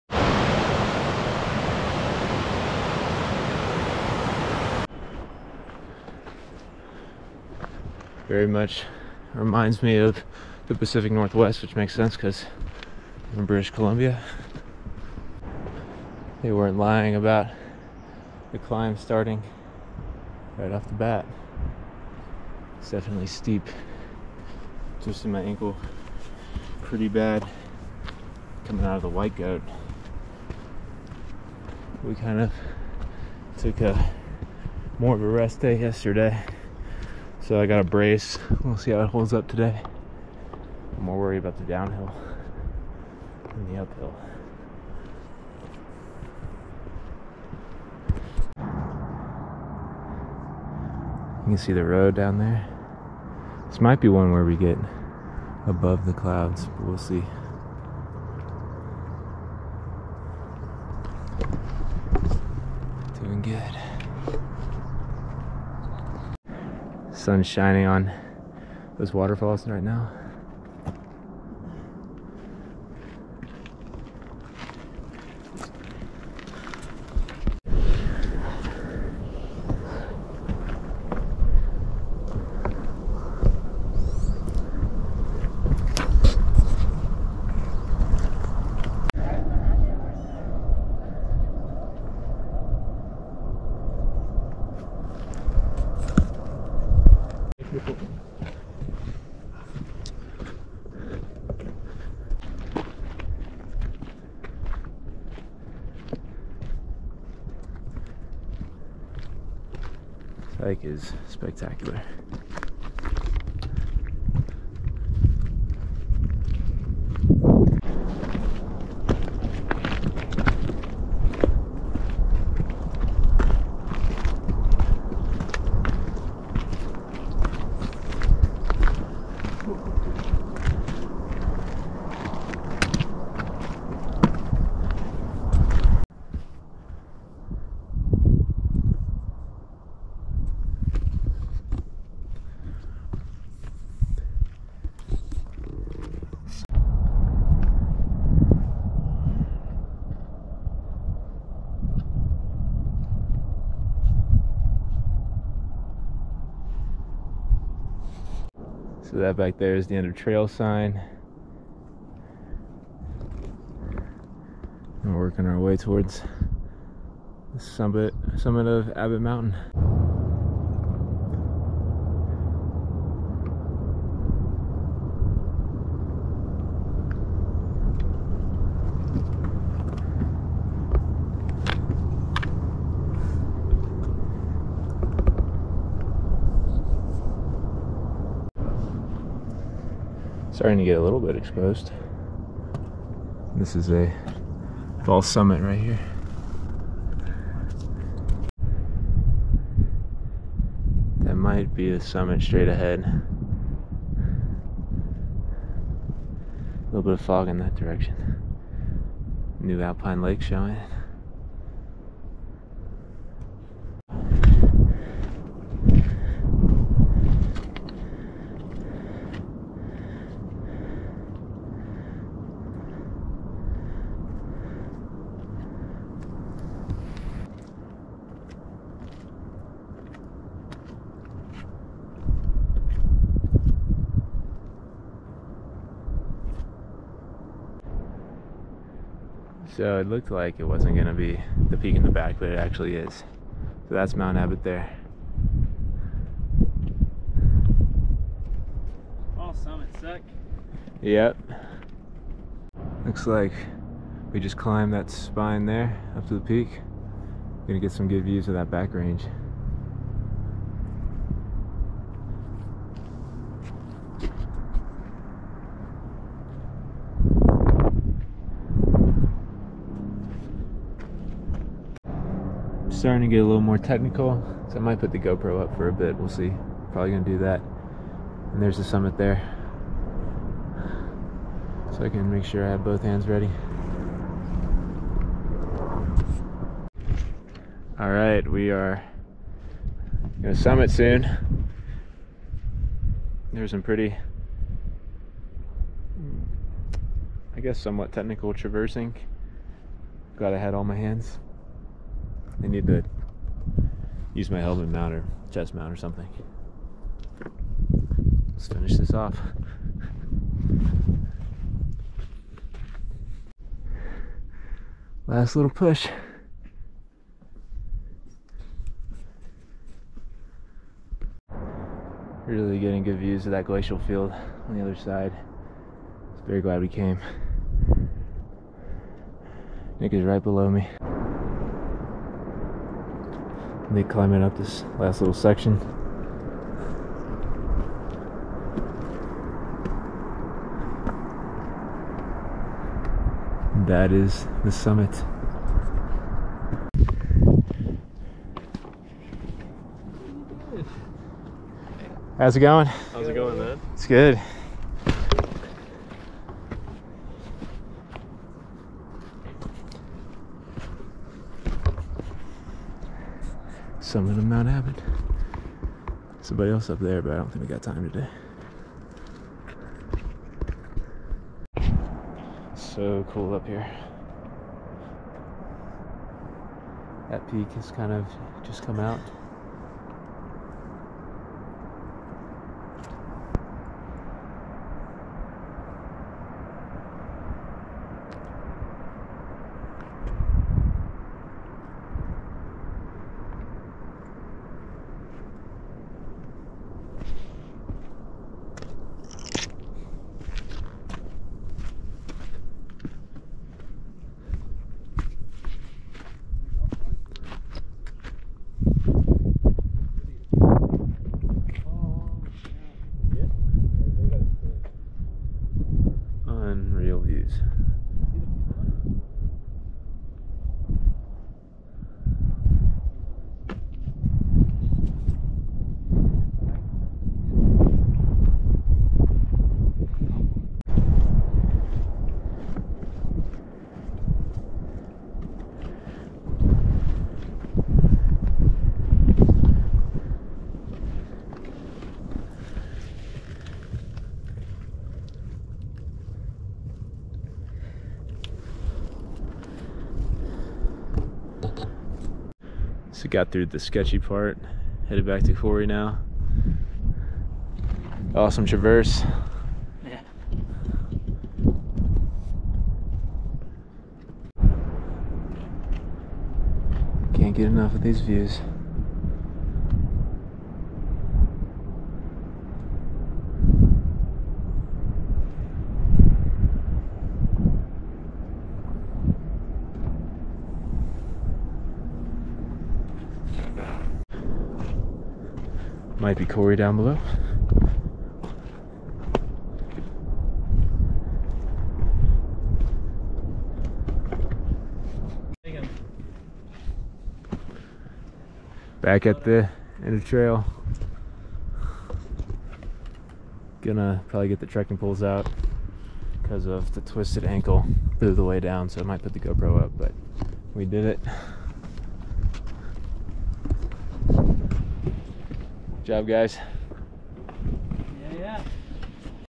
Mount Abbott: Glacier National Park of British Columbia
Mount Abbott was possibly my favorite hike of my recent trip to the Canadian Rockies and Montana. Glacier National Park of BC seems to be much less known than the one in Montana, but both are spectacular. The whole drive along Rogers Pass is truly incredible and I would highly recommend adding this to the list if you are taking a trip to the Canadian Rockies!